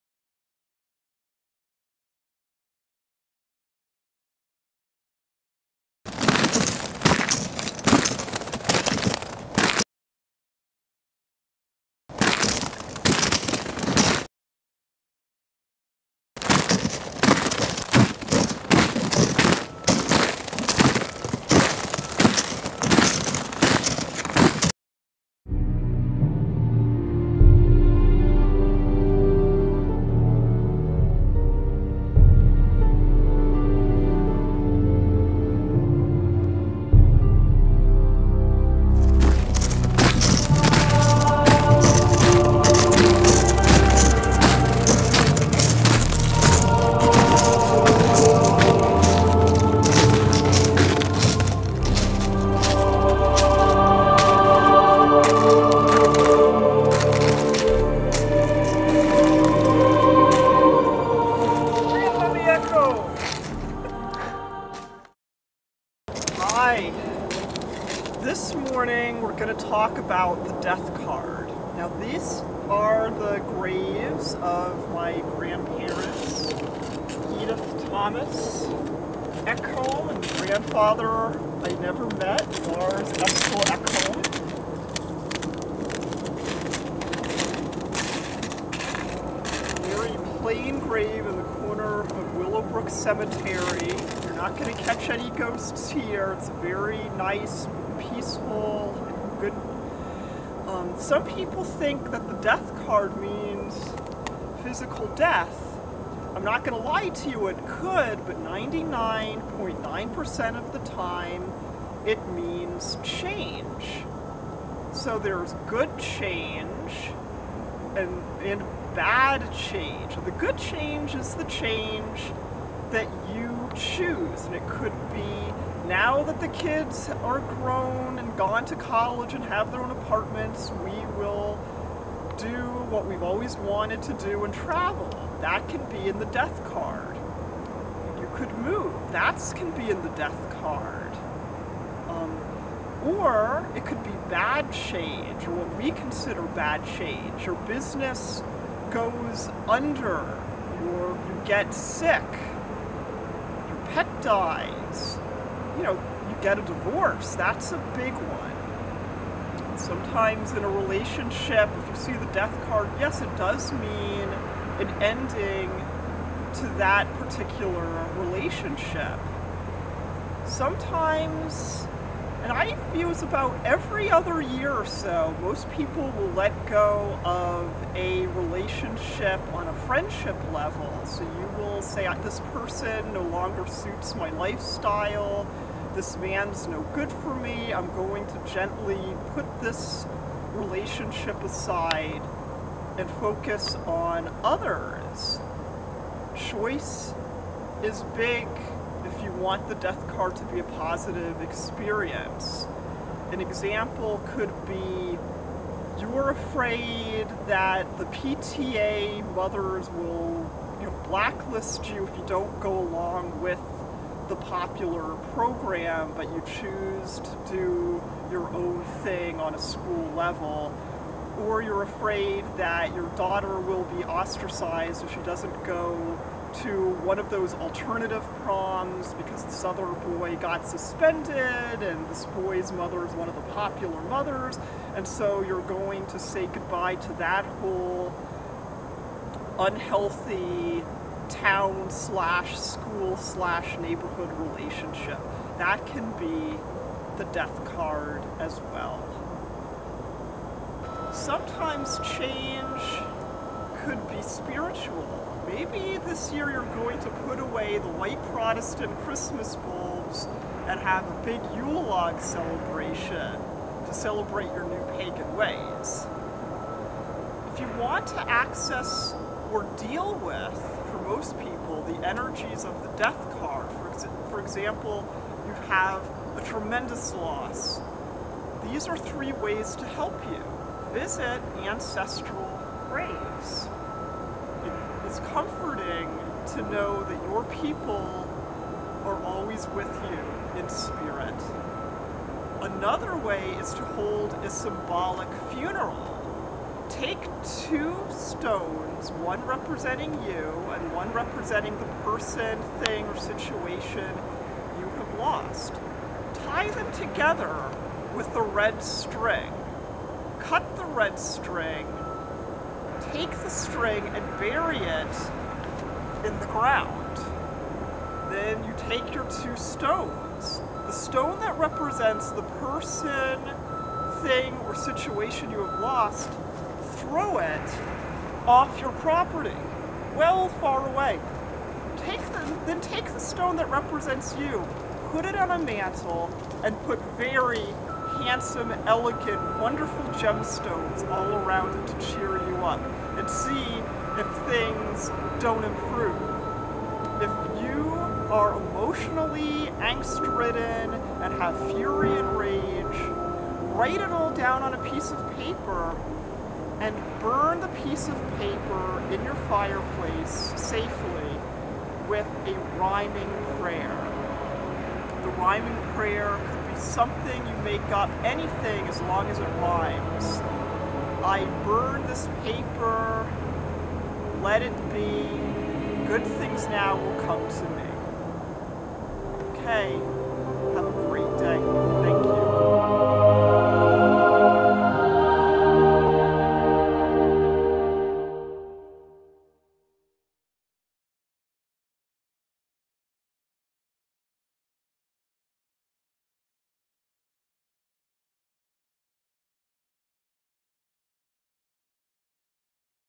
The Death Card
Aunt Crystal explains the Death Card and suggests ways to access (or avoid) the energies of this card.